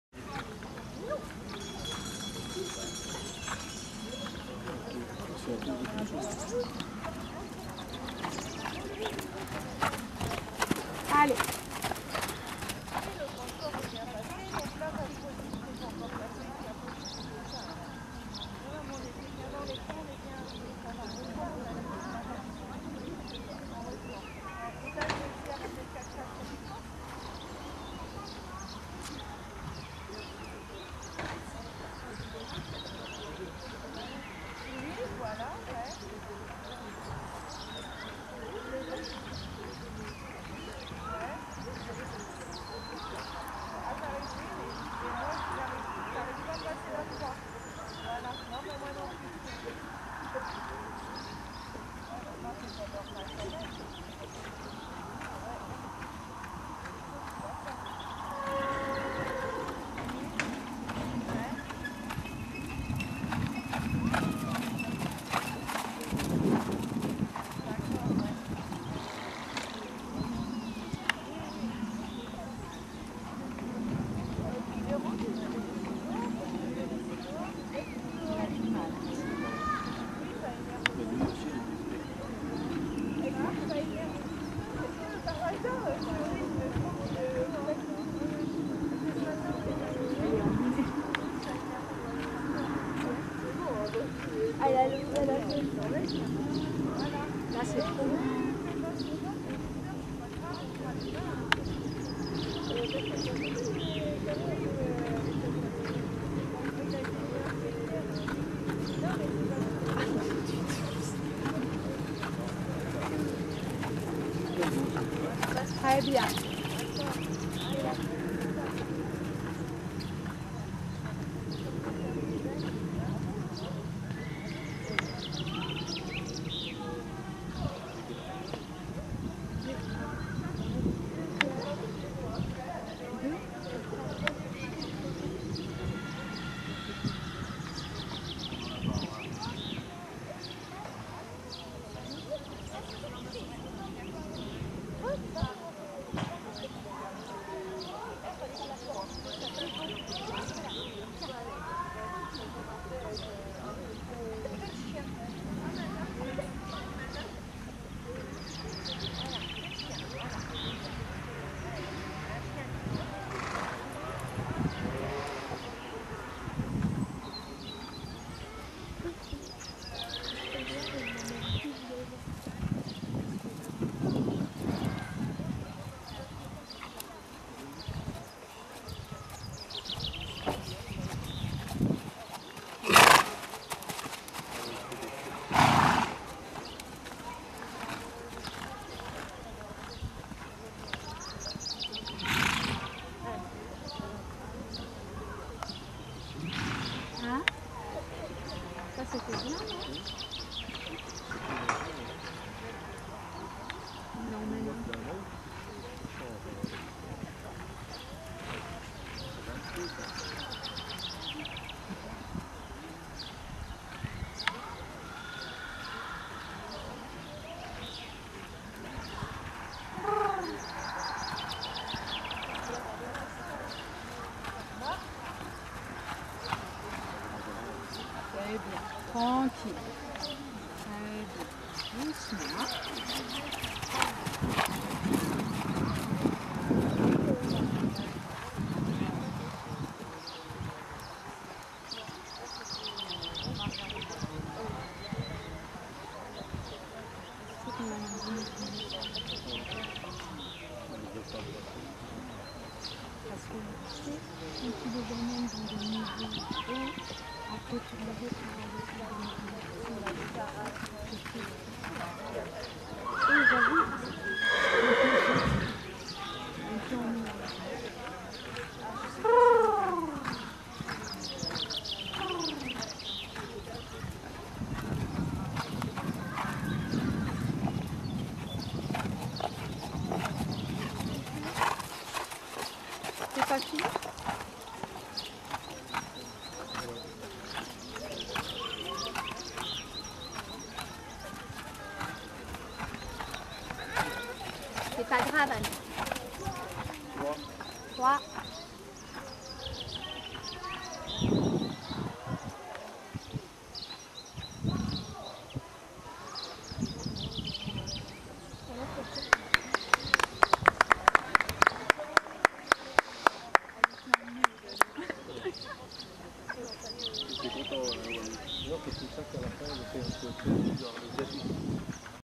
Anouck&Waou VillersEnOucheAmat3 20140316
C'était le 1er concours de dressage, à Villers en Ouche dimanche 16 mars 2014, de ce couple formé d'Anouck, jeune cavalière avec une petite expérimentée en concours et Waou qui n'avait plus vu de terrain de concours depuis 2012. Malgré quelques erreurs de la jeune cavalière, le résultat est prometteur : 1ère place au classement sur 9 partants avec 73,5 !!!